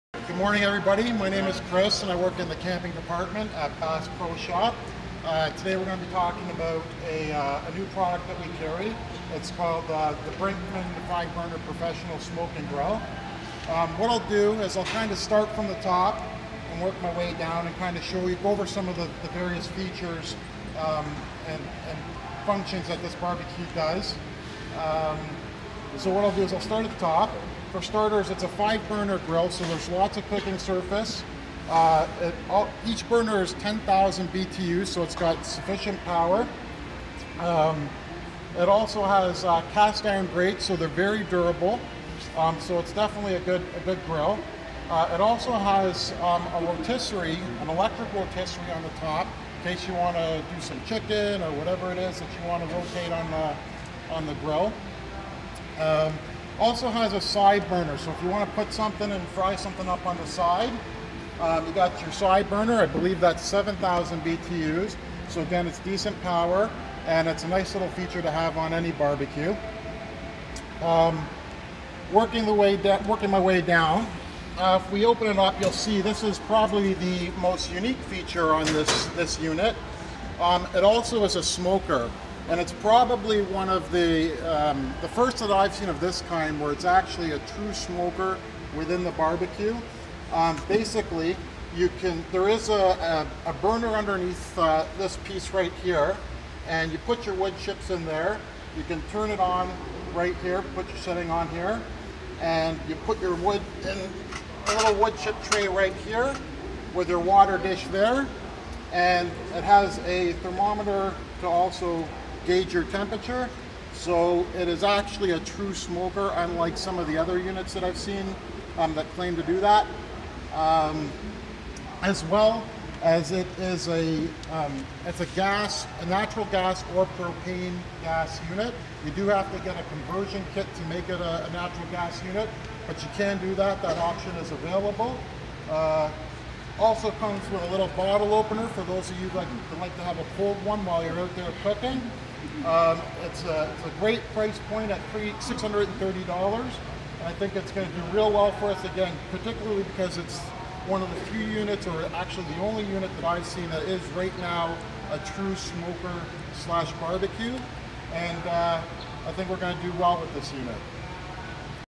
Brinkmann's 5-Burner Profession Smoke N' Grill
Chris from our Camping department shares with you some great features of this NEW smoker barbecue we have at our store!